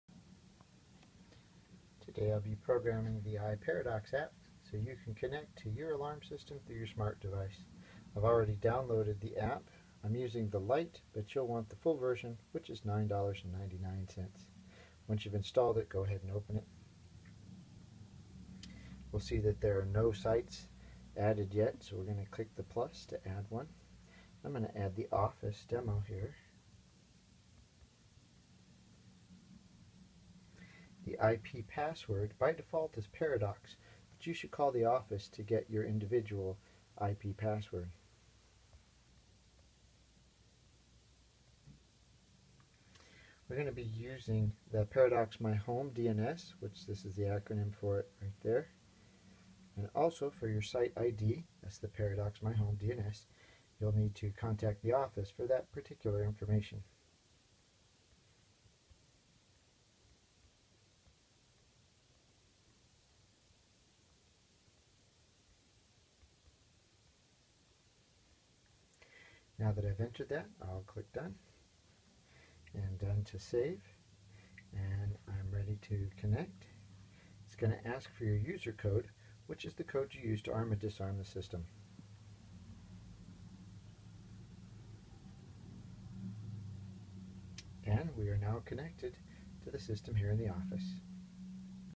Add your Paradox alarm system to the iParadox app.
Basic instructions to add the Paradox alarm system to the iParadox app.